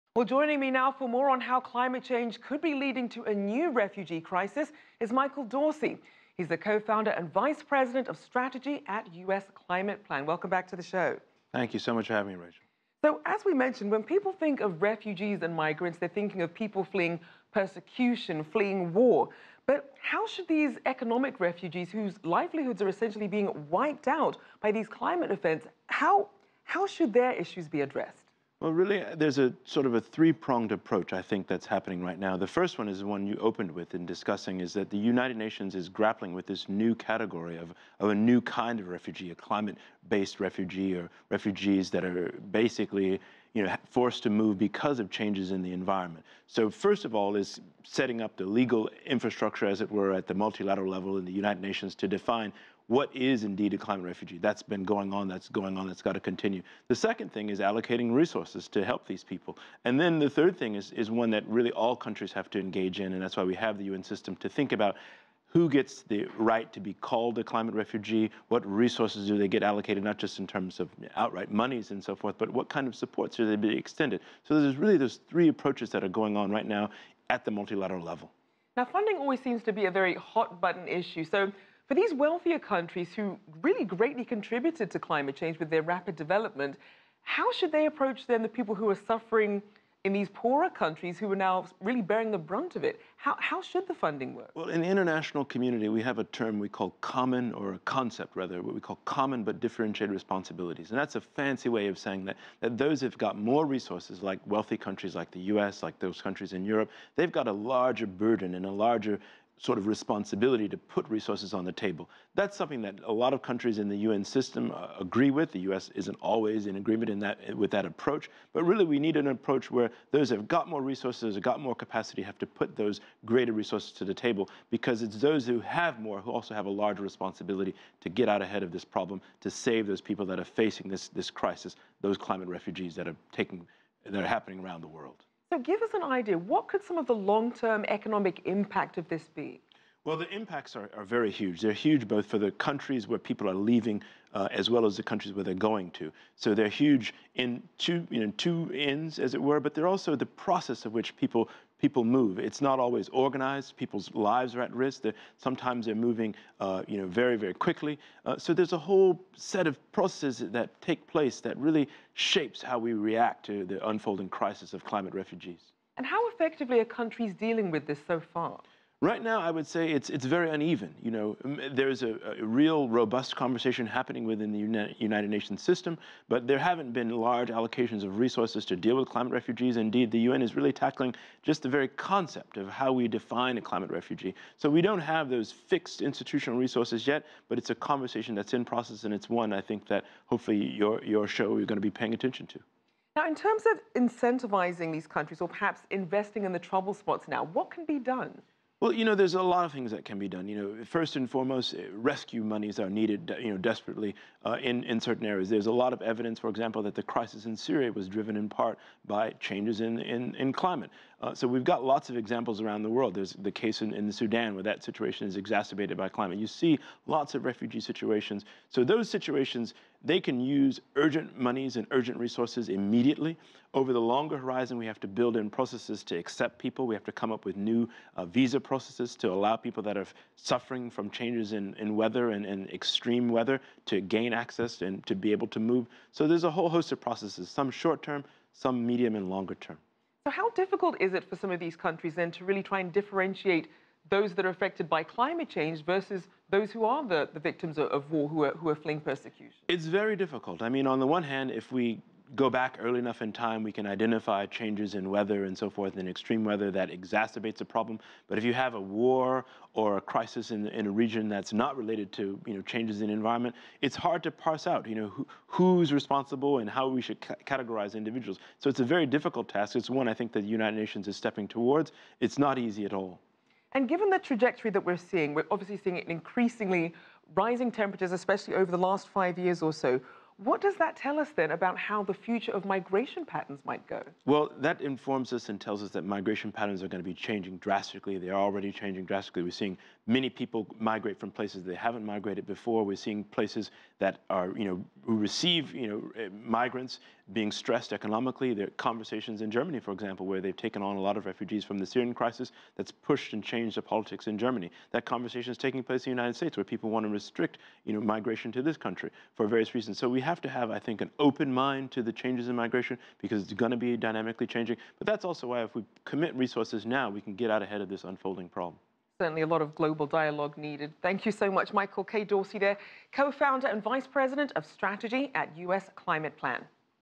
Michael Dorsey discusses the plight of environmental refugees
CGTN's Rachelle Akuffo spokes with Michael Dorsey, co-founder and vice president of Stretatey for US Climate Plan about the climate change and environmental refugees.